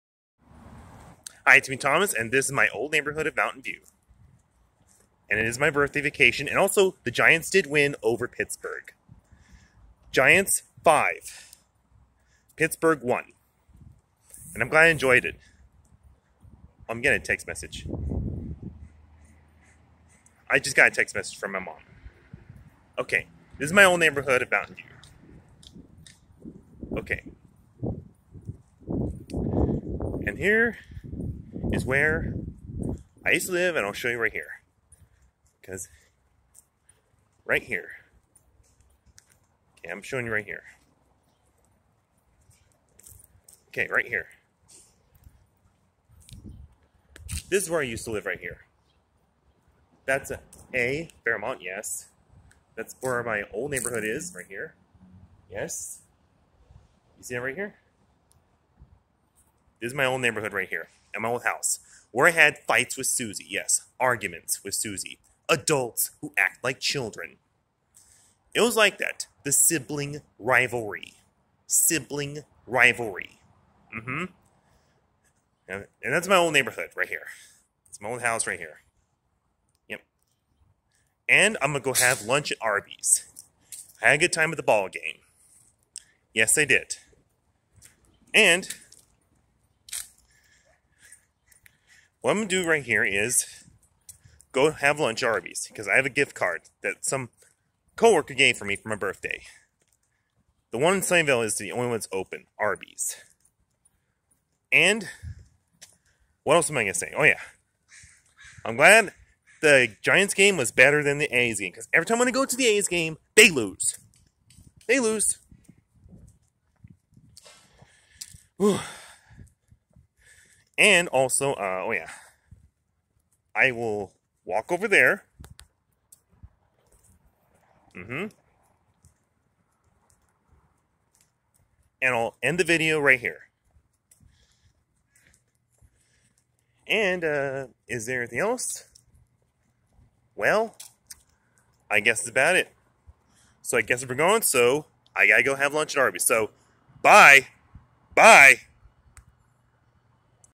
This is Thomas's old place where he used to lived. Had arguments with old sister Susie before died.
Thomas Murphy had a good time at the Giants game against Pittsburgh and Giants 5 Pittsburgh 1 so the Giants win that game but every time Thomas goes to the A's game, they lose. Thomas did show his old place where he used to live before he moved because Thomas had arguments with his old sister Susie which is Adults who act like children. It is this:sibling rivalry. Thomas will have lunch at Arby's because it is the only one in Sunnyvale and he got a gift card from his work. Next video is July 27.